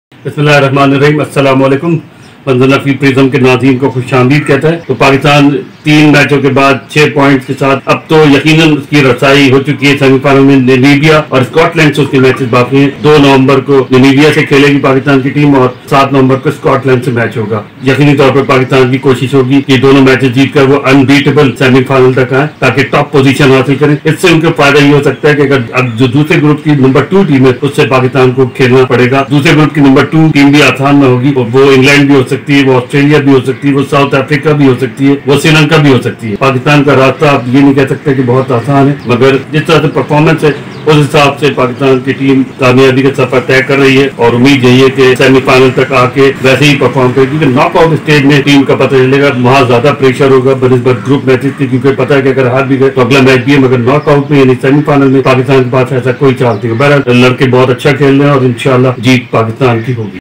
Pakistan Qualifies To Play T20 Semi Final | Pakistan Vs Namibia | Pakistan Vs Scotland | Predictions
Pakistan almost Qualifies To Play T20 Semi Final after beating Afghanistan. Pakistan Vs Namibia and Pakistan Vs Scotland matches are being played next. Here is the expert opinion about the matches and Predictions about the expected outcome.